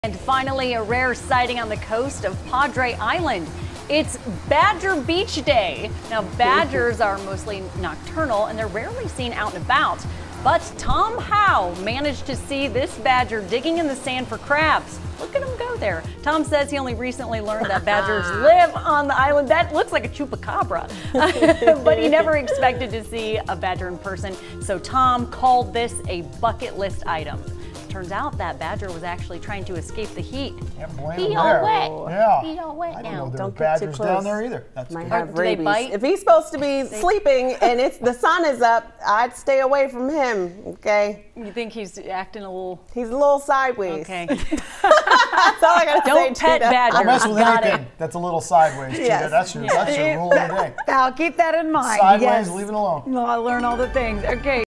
This elusive creature was spotted digging and swimming at a Texas beach
Officials say badgers can be found all over the state, including Padre Island